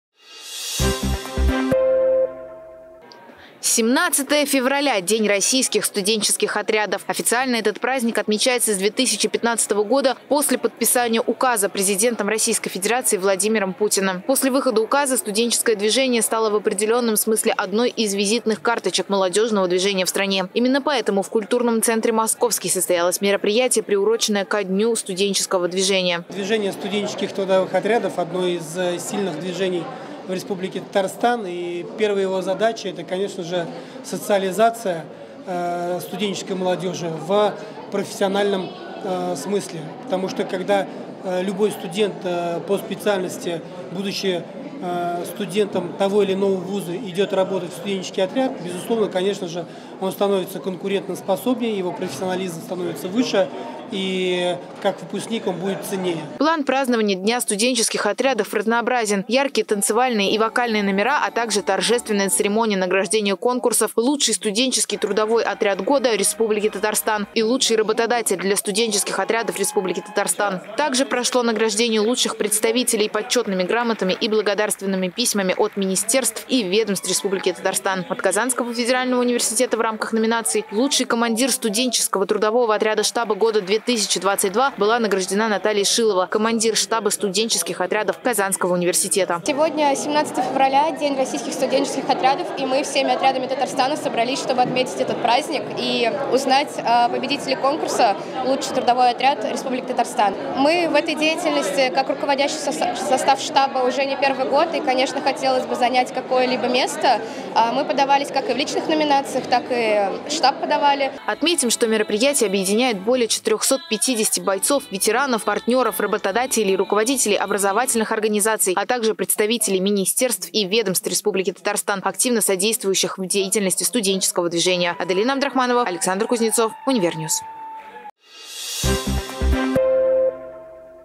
С ПРАЗДНИКОМ, СТУДЕНЧЕСКИЕ ОТРЯДЫ
17 февраля - День Российских студенческих отрядов.  О том, как этот праздник отметили студенческие отряды Казани расскажет моя коллега - Аделина Абдрахманова